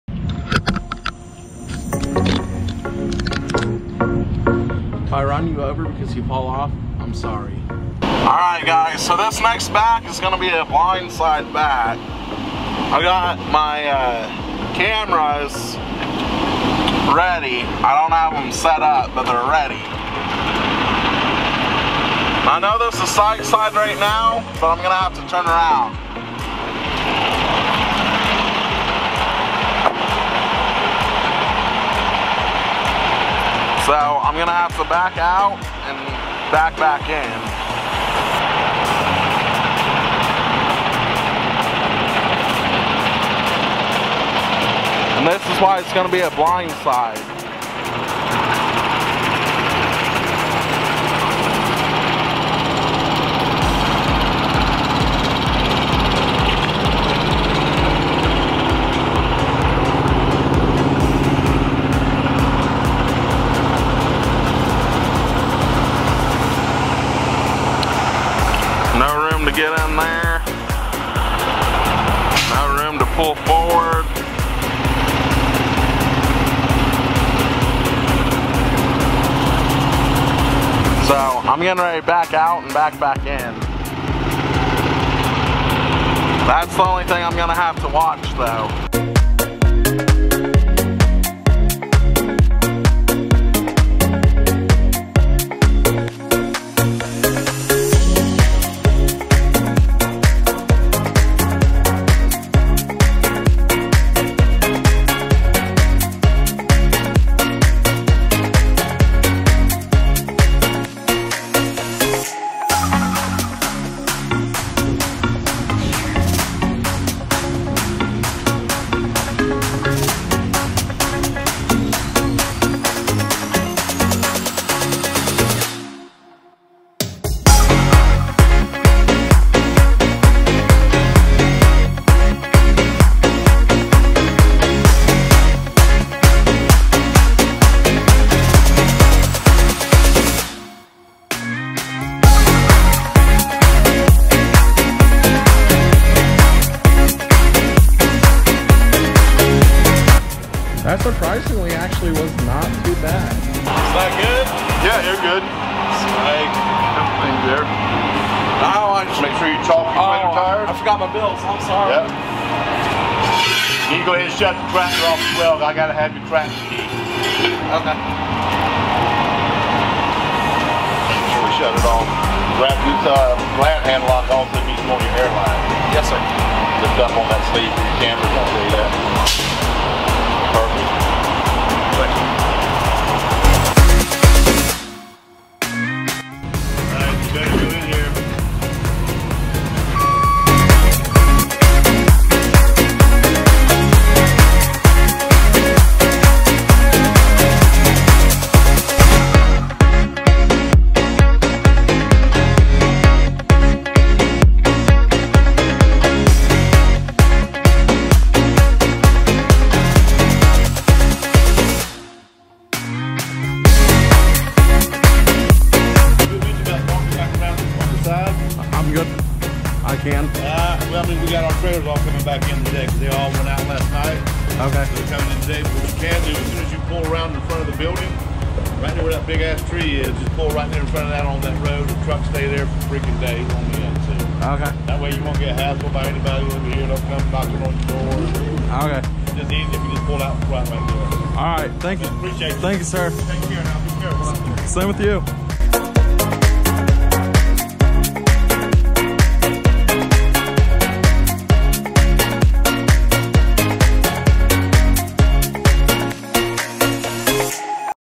The Coolest Receivers In Florida!
Today I got Live Unloaded In Florida (FL) and the receivers were the coolest. They let me film and I got some good shots! I'm excited to see what's next.